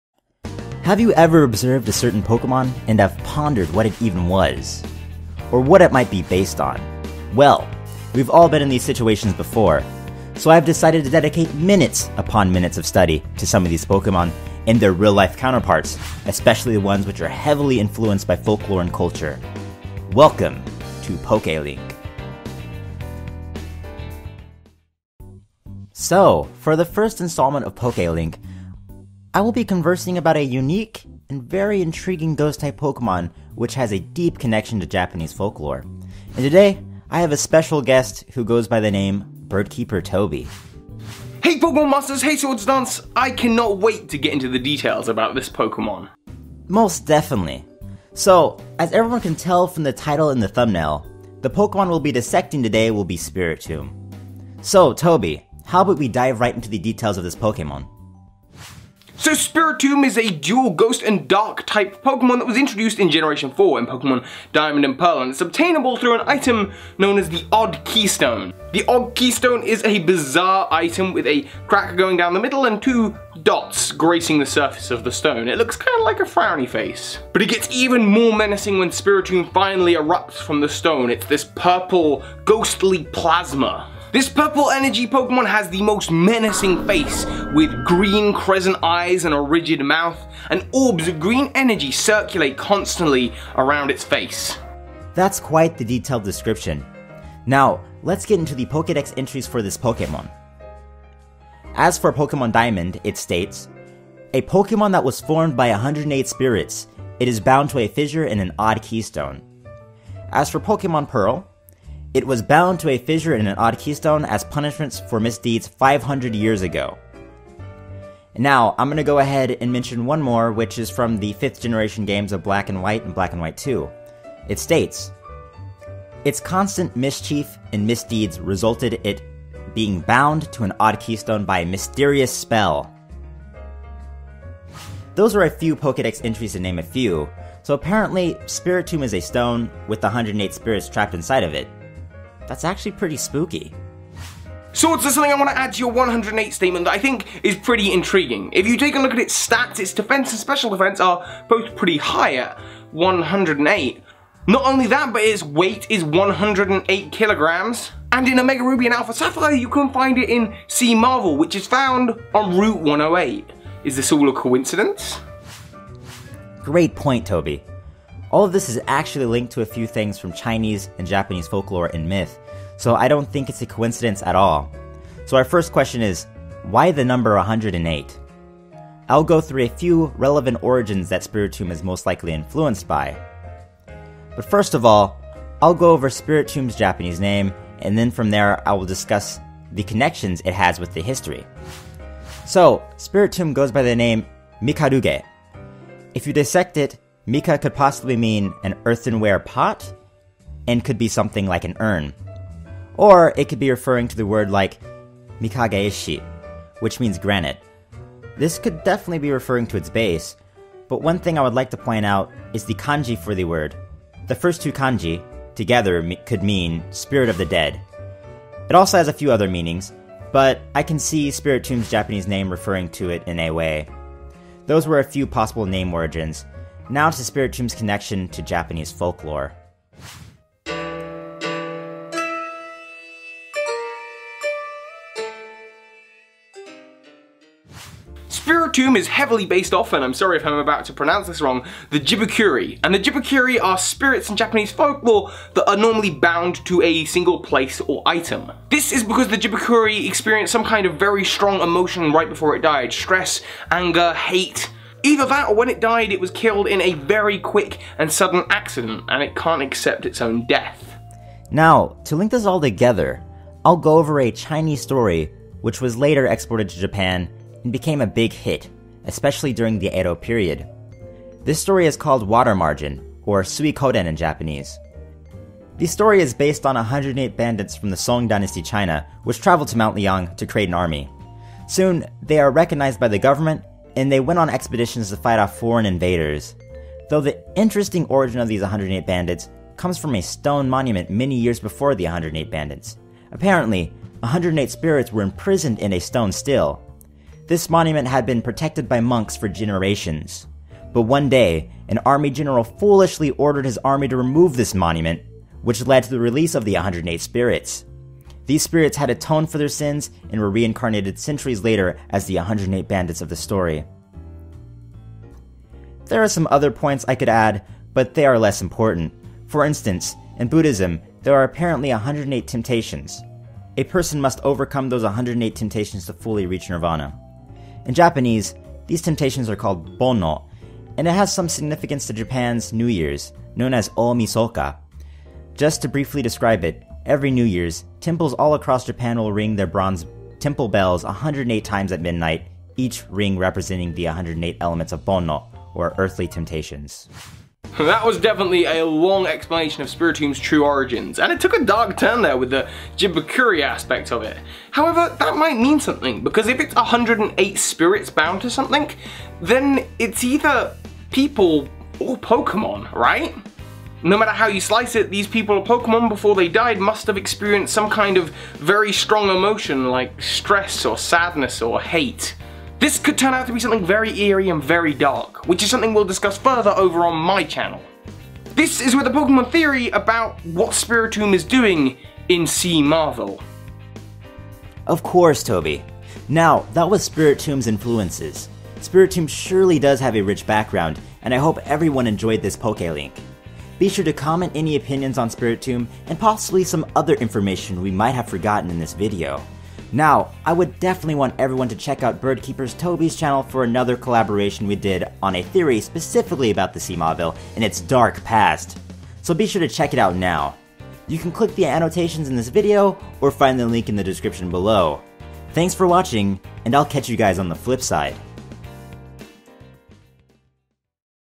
Poke-Link: Spiritomb's Origins
Yolo guise, welcome to Poke-Link, a new series where I conduct research on a certain Pokemon, and uncover their real-life counter parts and/or influences. In the first installment, I have a special guest joining me to talk about the Ghost/Dark type Pokemon, Spiritomb.